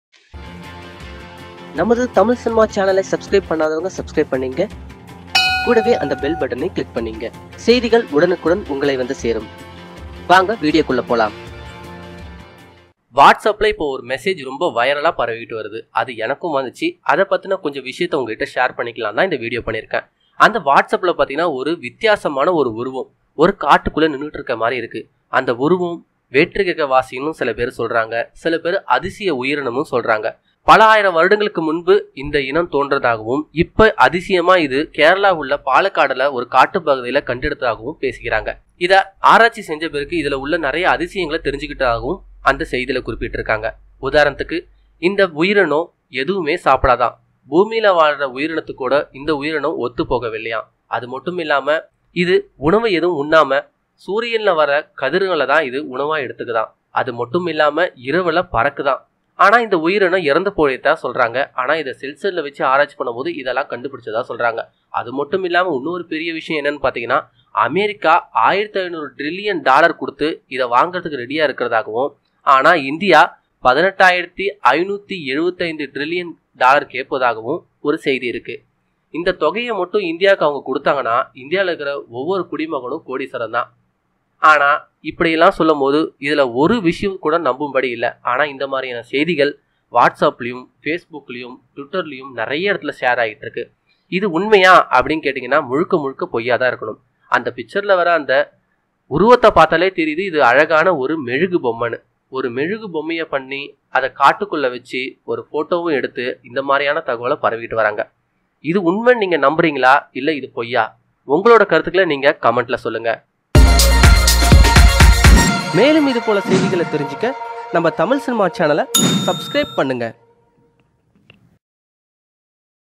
கேரளா நிலம்புரில் அதசிய உயிரினம்!!!
in kerala mistry face like human they called alien in forest side
soical media spread this image and telling is alien
is no eat food is eat only sun rise light and in night they will fly
like news in this case so i explaind about this